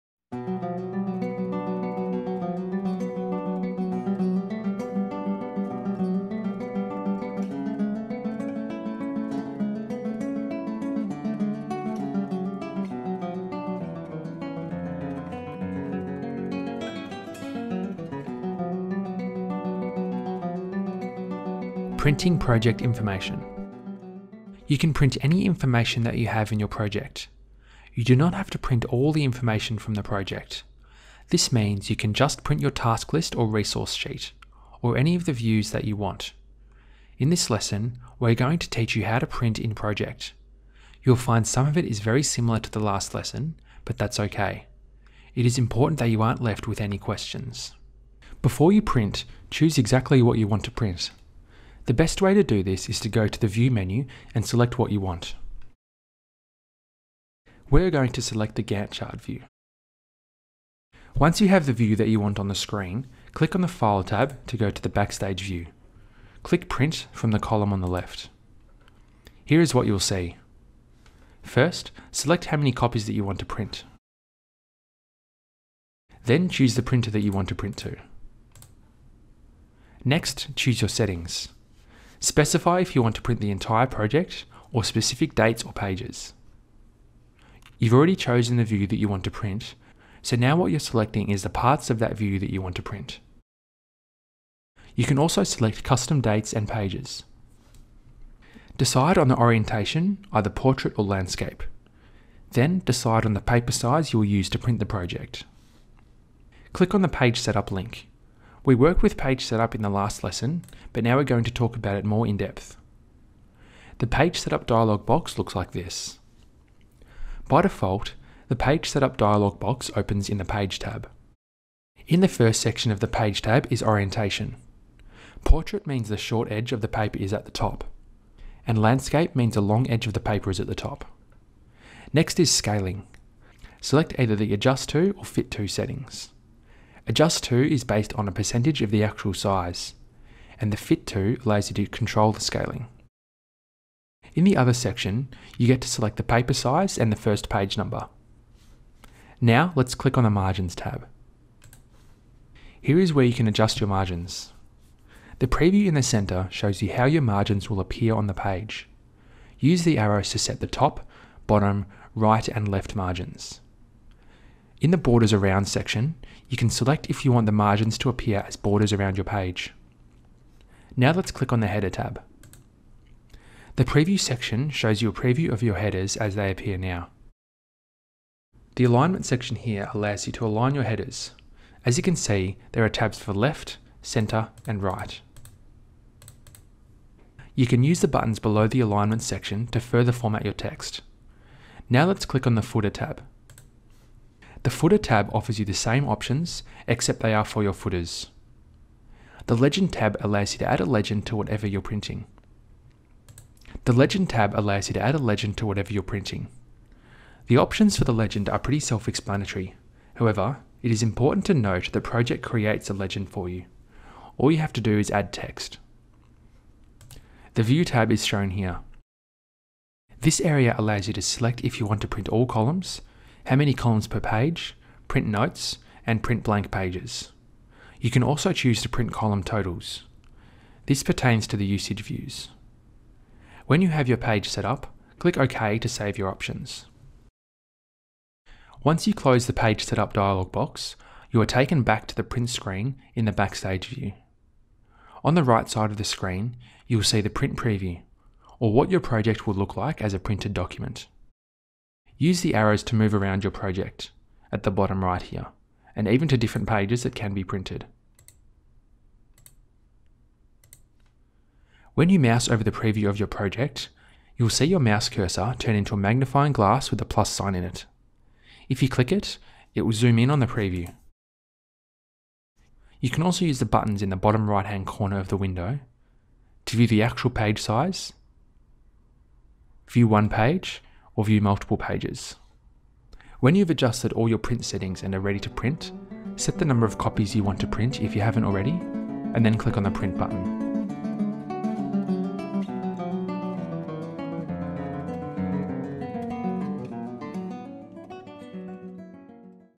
Lesson 18: Printing Project Information - Microsoft Project 2016 Course | Universal Class
Learn how to print out project information in Microsoft Project 2016.